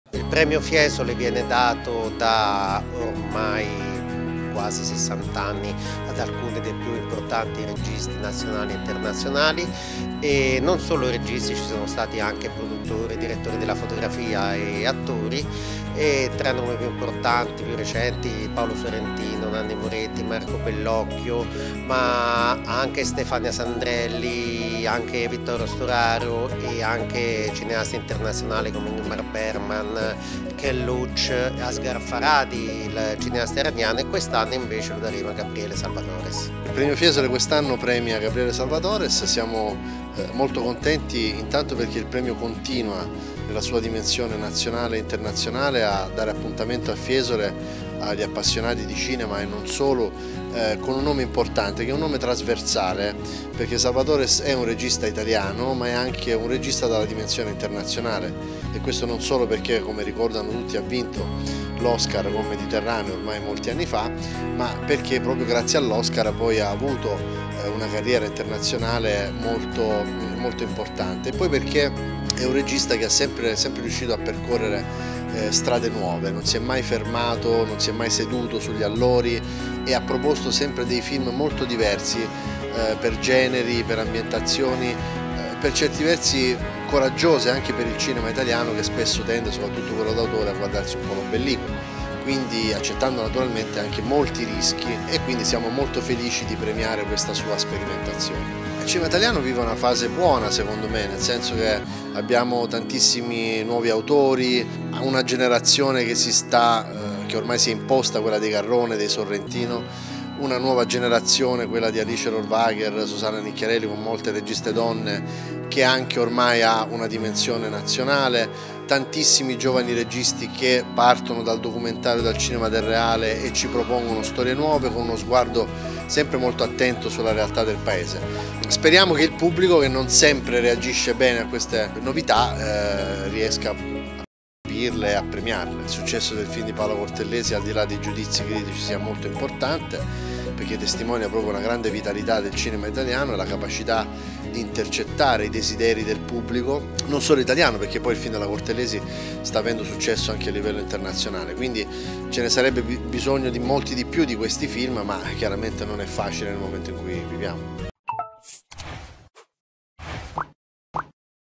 Gabriele Salvatores - Premio Fiesole 2024
Di Samuele Rovini

Andrà al regista Gabriele Salvatores il Premio Fiesole ai Maestri del Cinema edizione 2024, prestigioso riconoscimento conferito dal Comune di Fiesole in collaborazione con il Sindacato Nazionale Critici Cinematografici Italiani Gruppo Toscano e la Fondazione Sistema Toscana, per la direzione artistica di Simone Emiliani.

La serata di premiazione di sabato 29 giugno si svolgerà al Teatro Romano di Fiesole (Largo Fernando Farulli, 1) con ingresso gratuito. Si aprirà alle 20.45 con un incontro con Salvatores e la presentazione del volume monografico, dal titolo “La realtà immaginata. Il cinema di Gabriele Salvatores” a cura di Paola Casella con i contributi dei soci del Sncci, per le Edizioni ETS di Pisa. Ospite speciale della serata l’attore Fabrizio Bentivoglio che ha lavorato con Salvatores in Marrakech Express (1989); Turné, (1990); Puerto Escondido (1992); Denti (2000); Happy Family (2010); Il ragazzo invisibile (2014) e il recente Il ritorno di Casanova (2023). La cerimonia di premiazione si terrà alle 22 e a seguire sarà proiettato il film Tutto il mio folle amore, del 2019, che vede protagonista Claudio Santamaria.
 Il Premio Fiesole ai Maestri del Cinema è realizzato grazie al contributo della Regione Toscana e della Fondazione CR Firenze e con il sostegno di Villa San Michele, A Belmond Hotel, Florence.